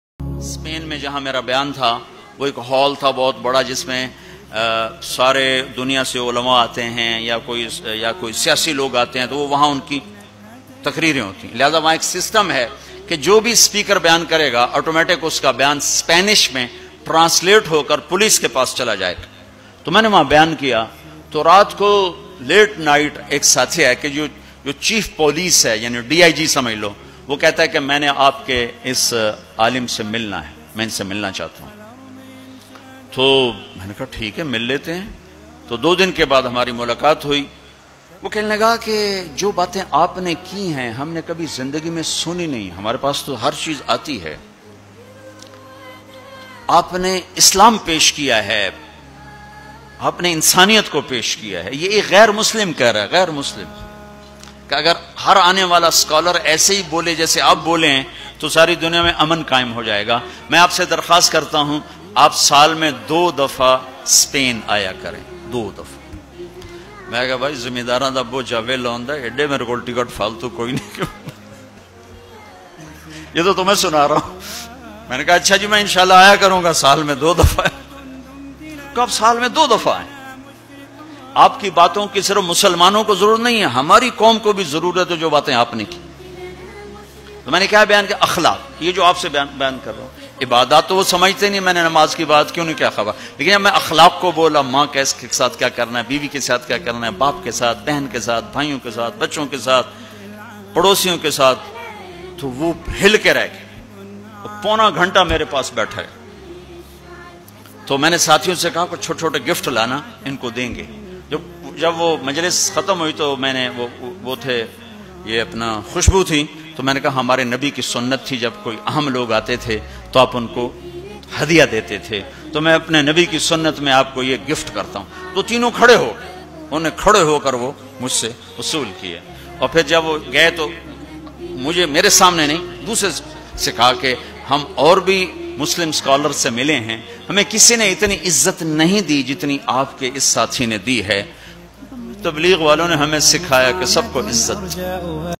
سپین میں مولاناصاحب کابیان سن کرسپین کے پولیس آفیسر متاثرھوکرمولاناصاحب سےملنے کی خواٸیش کی سنیۓ زبر
سپین میں مولاناصاحب کابیان سن کرسپین کے پولیس آفیسر متاثرھوکرمولاناصاحب سےملنے کی خواٸیش کی سنیۓ زبردست بیان سن کرشیٸر کریں